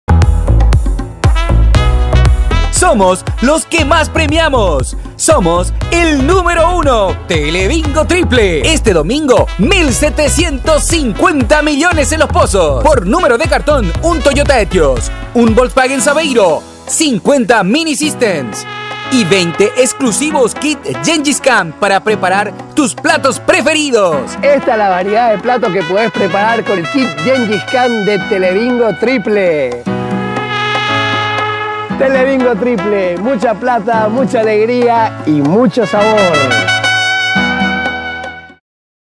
Telebingo Triple - comercial 20-09-15
Somos los que más premiamos
Somos el Número 1, Telebingo Triple.

Este domingo 1750 millones en los pozos

Por número de cartón
1 Toyota Etios
1 VW Saveiro
50 Mini Systems

Y 20 exclusivos Kit Gengis kan para preparar tus platos preferidos

Telebingo triple,
Mucha plata. Mucha alegría